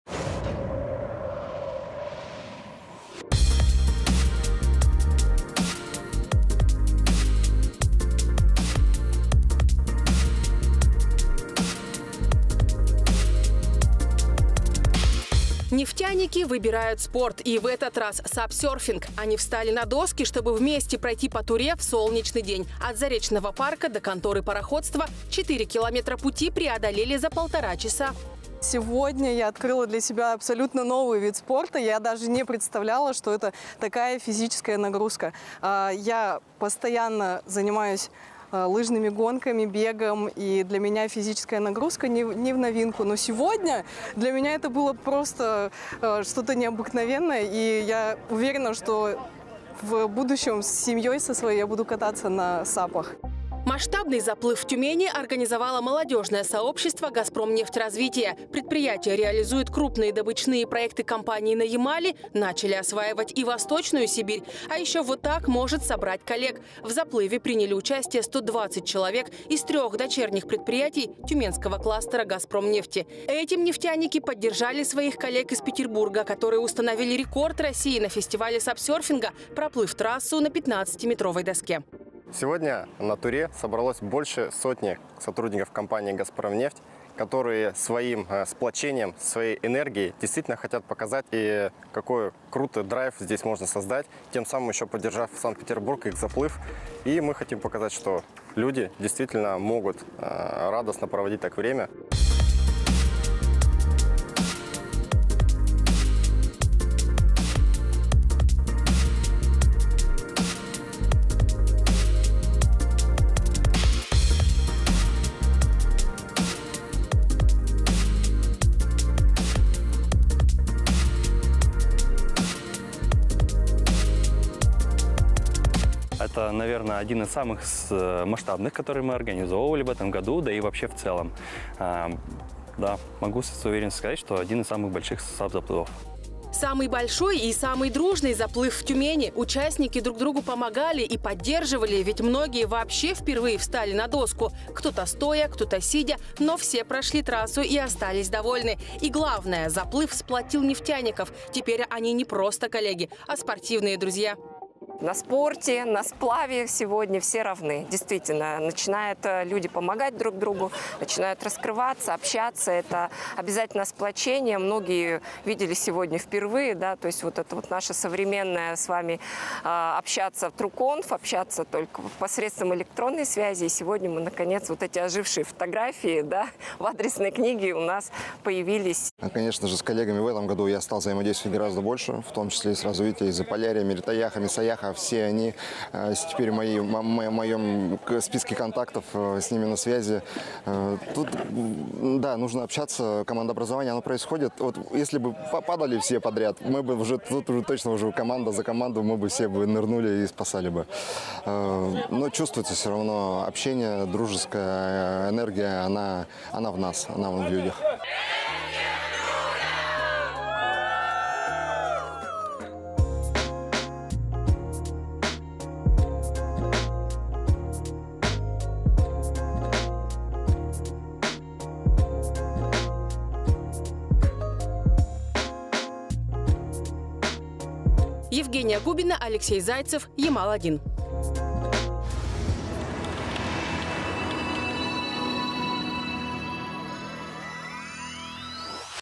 Встали на доски: в Тюмени прошел масштабный заплыв на сапах
Здоровый образ жизни – приоритет для сотрудников «Газпром нефти». Специалисты занимаются спортом и принимают активное участие в различных мероприятиях. Поэтому на заплыв на сапах заявилось большое количество желающих. Многие впервые встали на доску. Не у всех получилось с первого раза, но коллеги помогали и поддерживали. Ведь они – большая дружная команда.

Как прошел один из самых больших заплывов на Туре – смотрите в нашем материале.